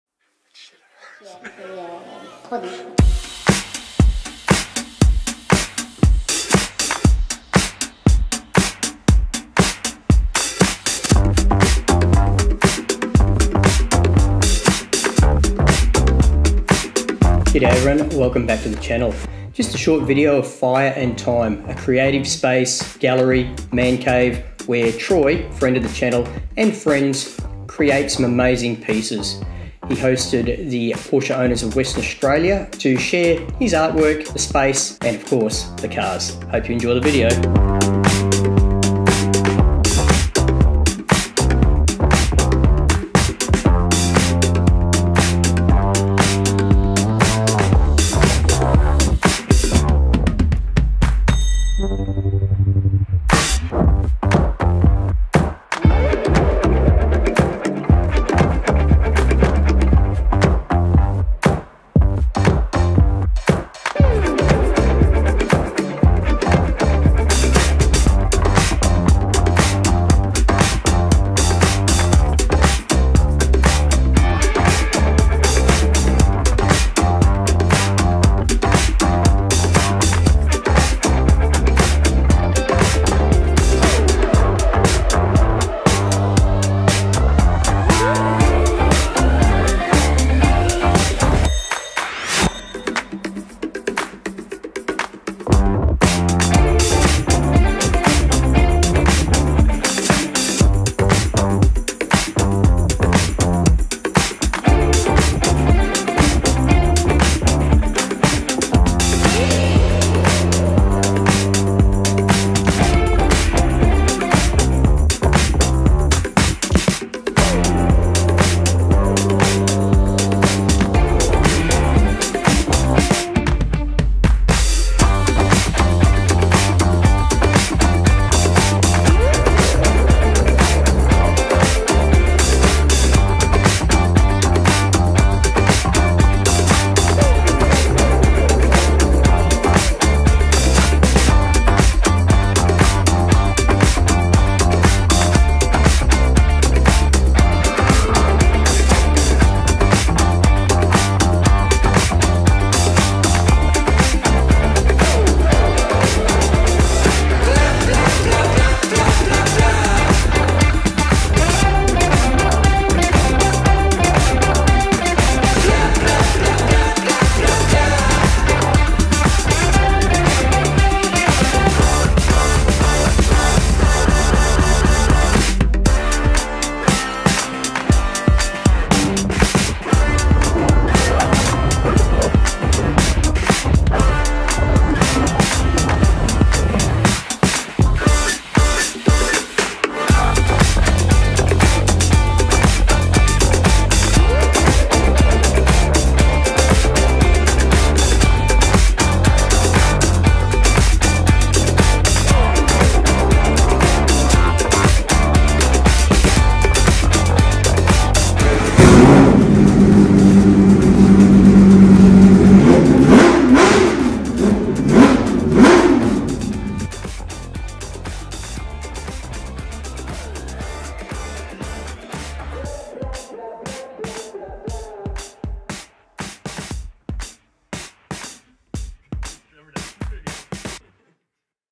Porsches and Art
Fire & Time hosts a Sunday Morning of enthusiasts from P.O.W.A.
Porsche Owners of Western Australia

Amazing art, cars, Motorcycles, watches, creative space, man cave.
Troy has lifted the bar. Thank you for hosting.

Marc and Cars – Auto-Media